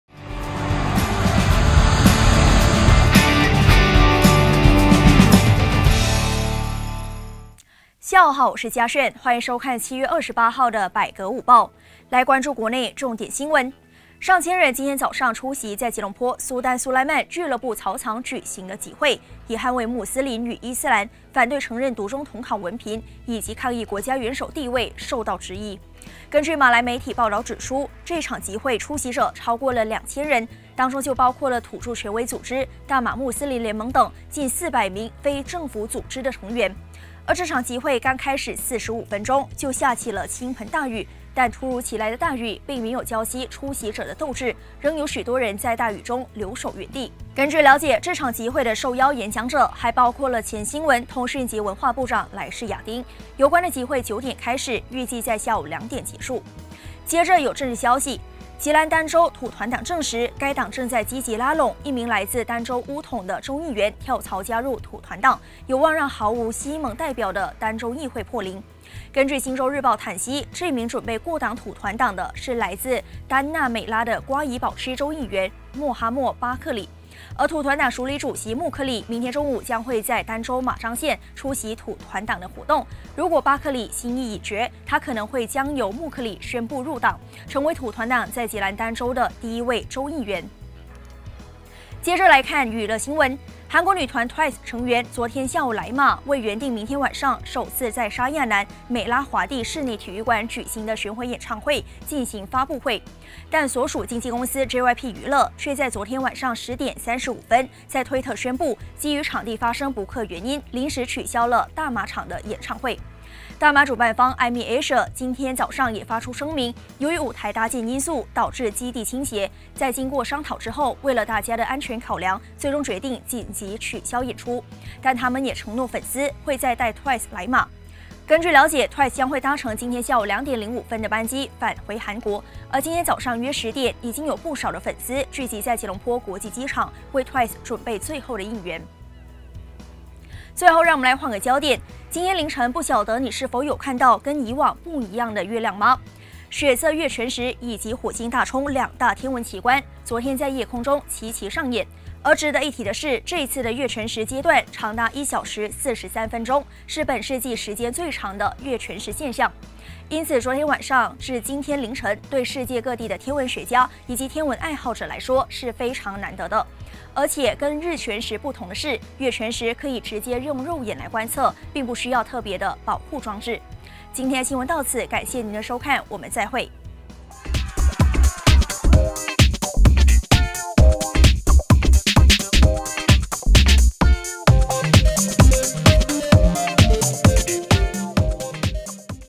昨晚取消今晚演出！主办方承诺：再带TWICE来马
退票补偿不了粉丝的心碎！